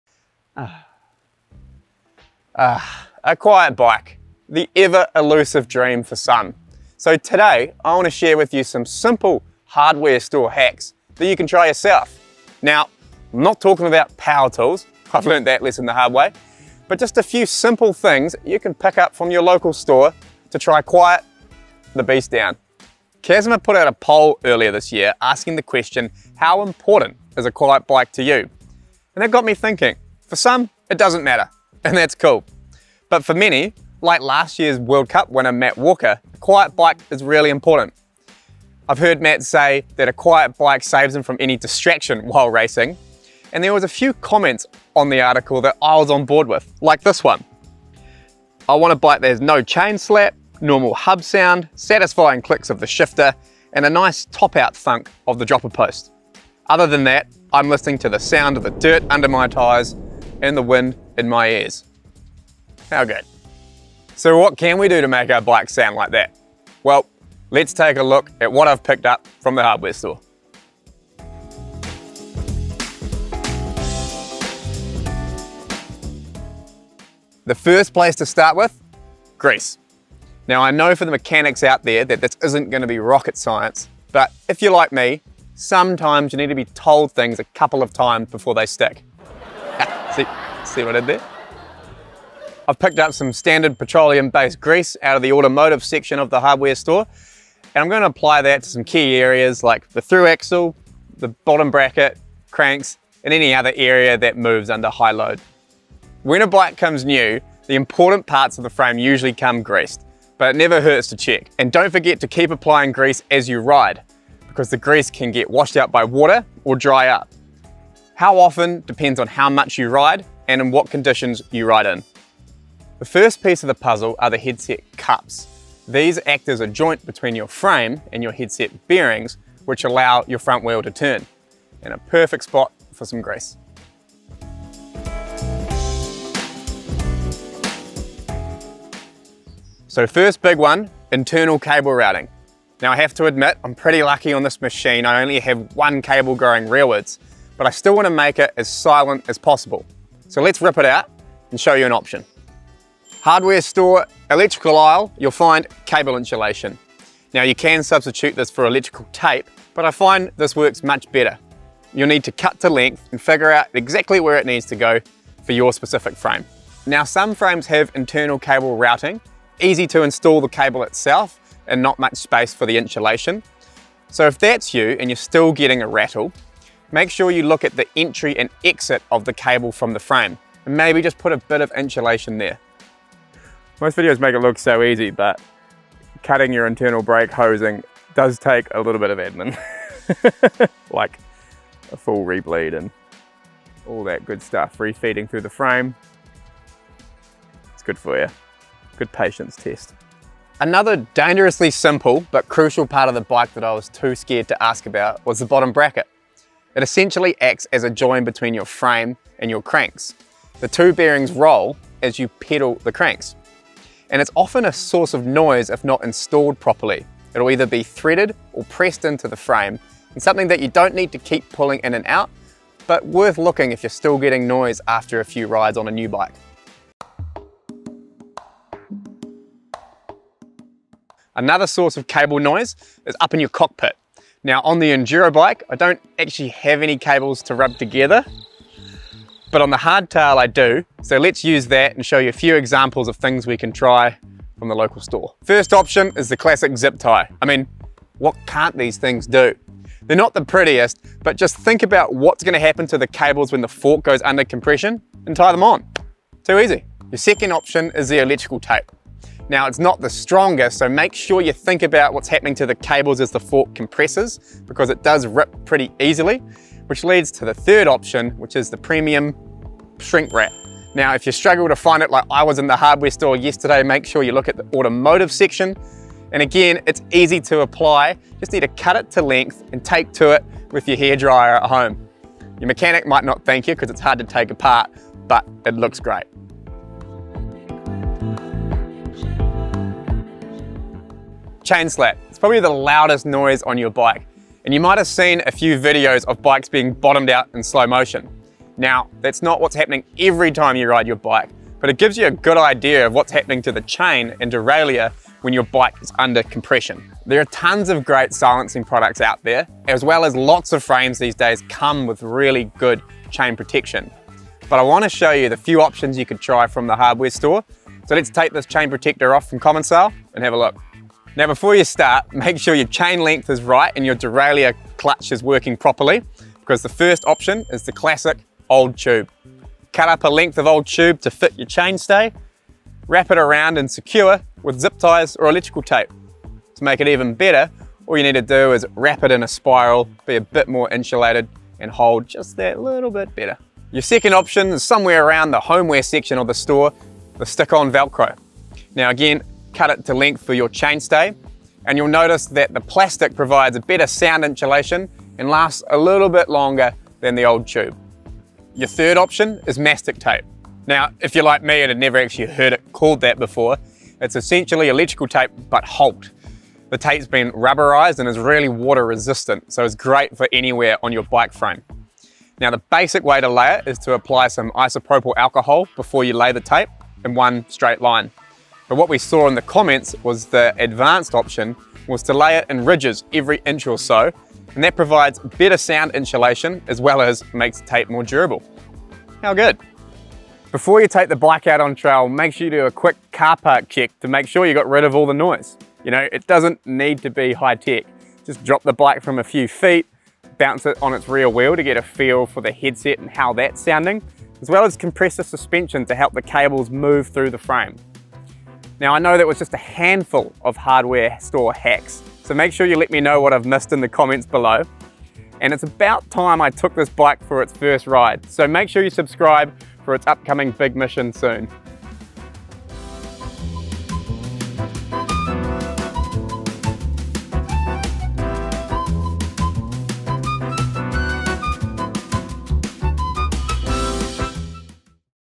Silence Your Bike With These Hardware Store Hacks
Shhhh..you hear that? Yeah there's a lot of noise out there, but your MTB doesn't have to be one of them. Tom Bradshaw helps you save some dollars by making sense of typical items you'd find in a hardware store, and how they can help quiet your rig down!

0:00 - Intro
01:10 - Grease
02:14 - Internal Cables
03:20 - Bottom Bracket
03:47 - External Cables
04:47 - Chain Slap
06:30 - Outro